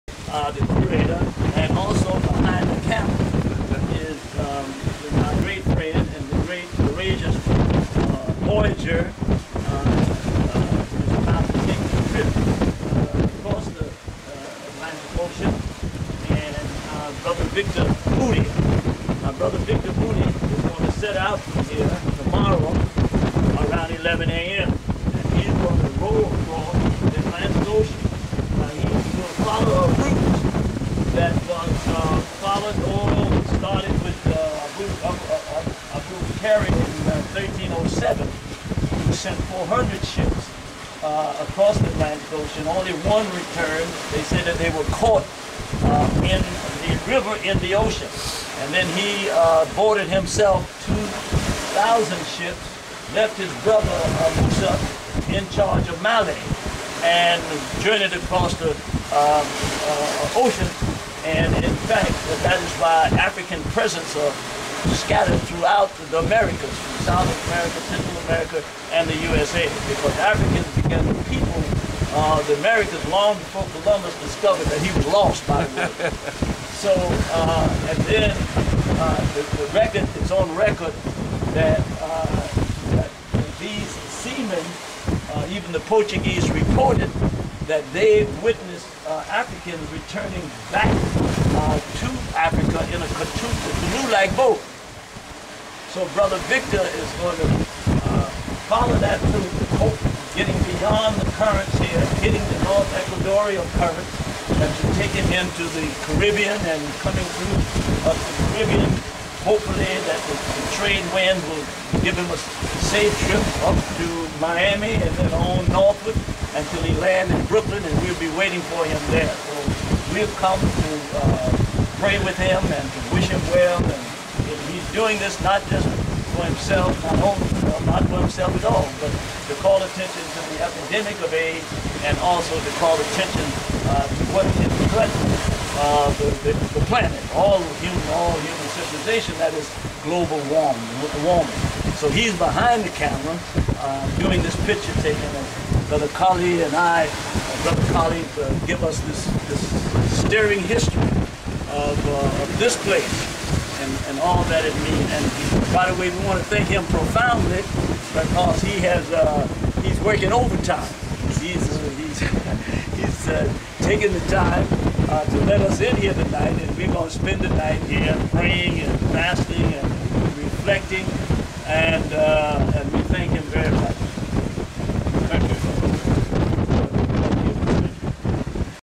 Slave House - Goree Island
(March 2009) Rev. Daughtry, Pastor of the House of the Lord Church in Brooklyn, New York and Victor Mooney listen to the slave house curator, Mr. Coly. Later, Rev. Daughtry talks about the journey that awaits Mooney